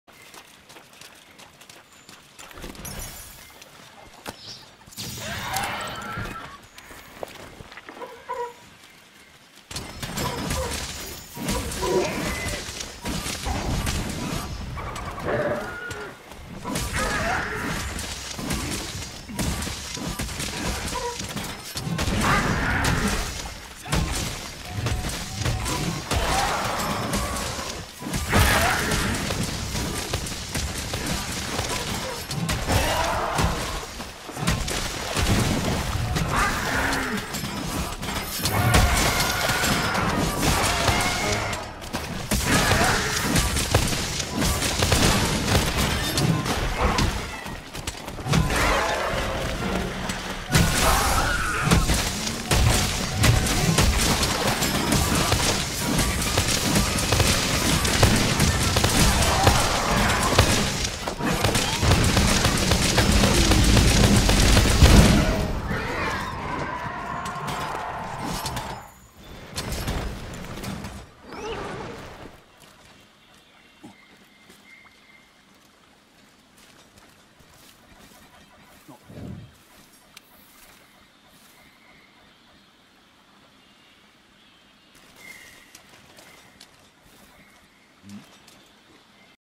Finally nailed the swaxe finisher
Made a new fashion set and wanted to test it out so I went on a expedition and started slaying shit. Getting the kill with the final blow of the switch axes elemental discharge is tricky to time but got it this time. Really fun build. The kulu was a full hp that was the first time I attacked it in the expedition.